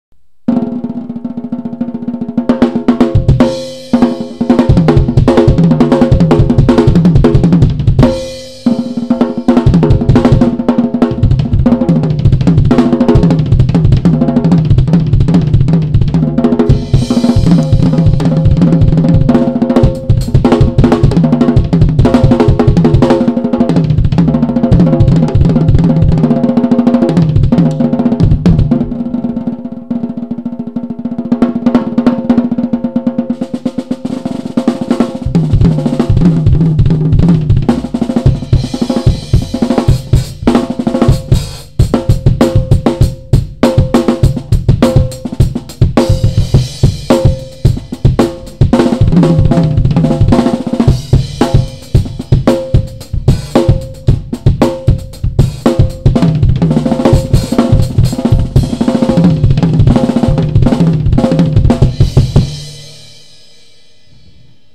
Drum Solo...(Single Bass Pedal)
This was really random but i was bored, I just decided to record audio but i didnt turn out as bad as i thought it was going to turn out haha :  ]